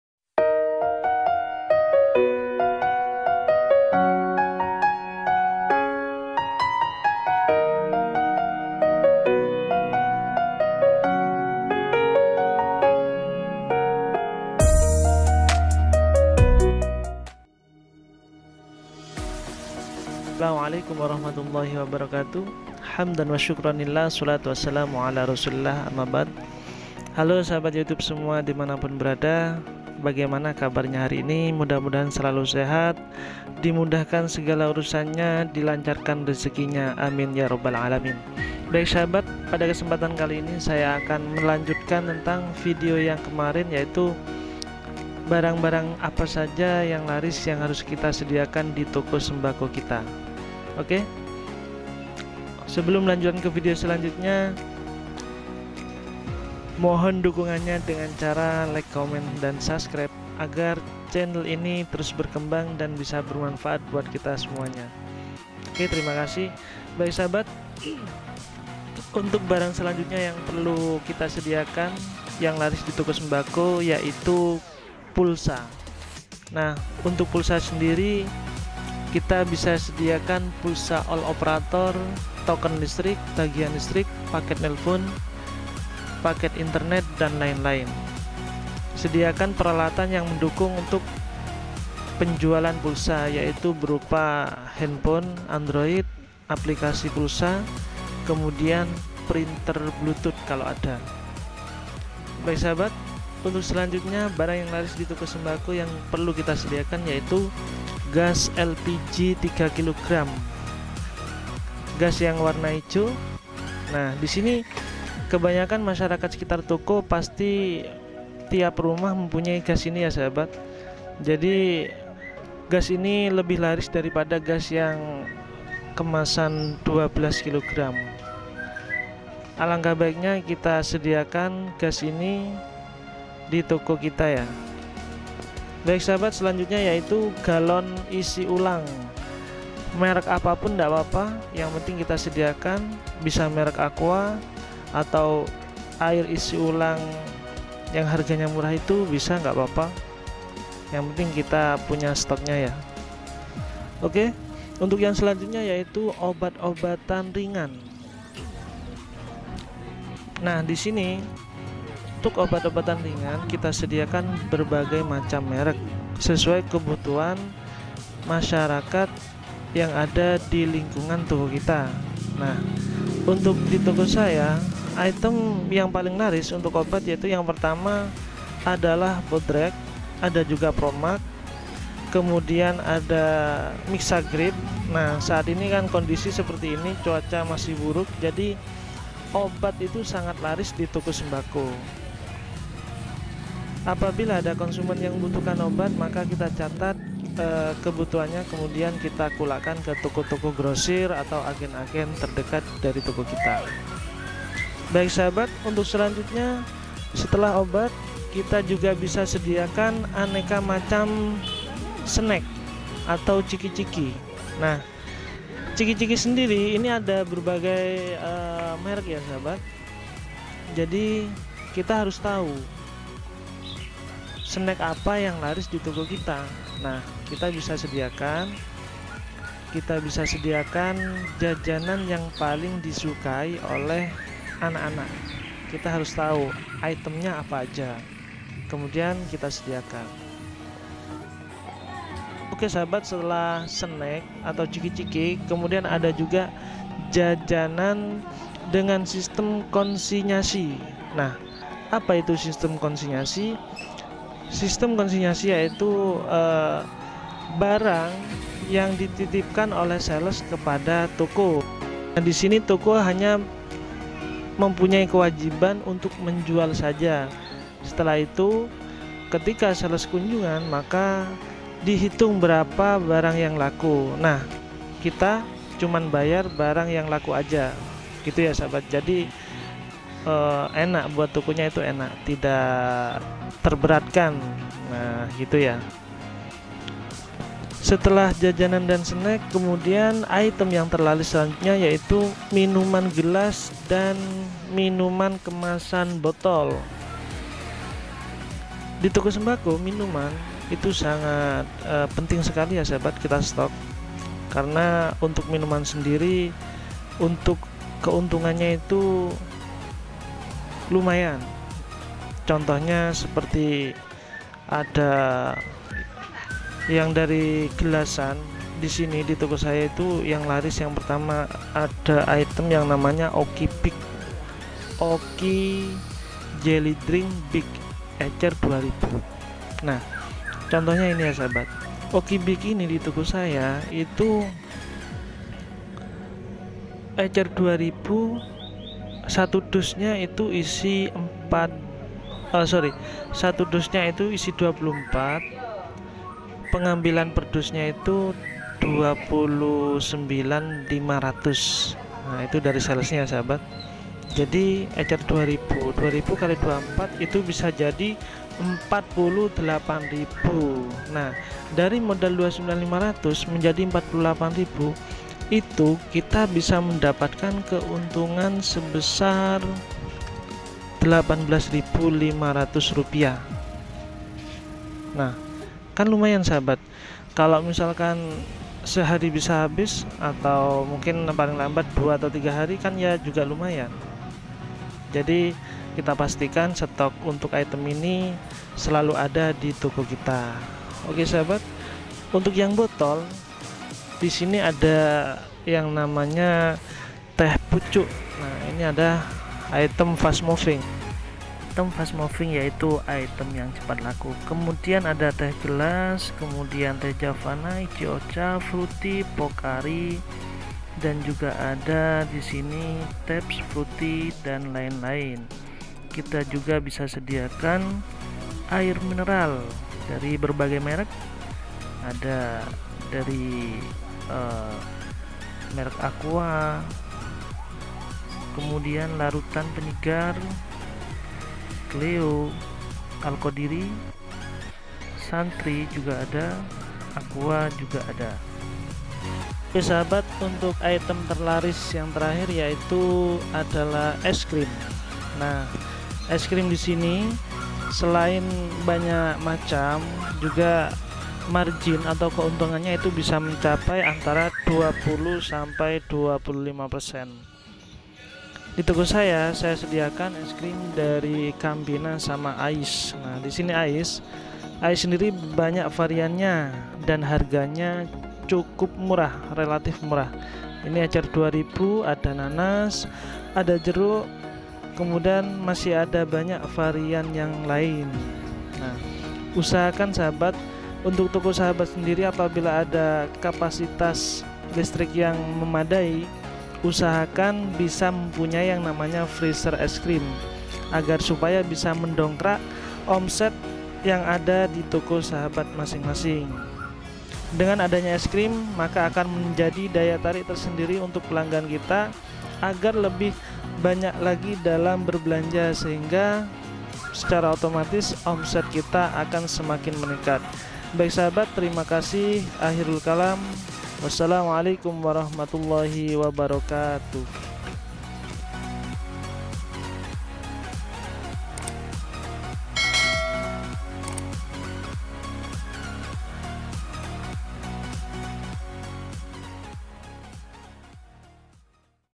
BARANG YANG PALING BANYAK DICARI DITOKO SEMBAKO ,Part2
Halo sahabat retail semua...
Konten ini adalah lanjutan dari barang yang paling laris ditoko sembako, semoga bermanfaat dan selamat menonton.
Salam sukses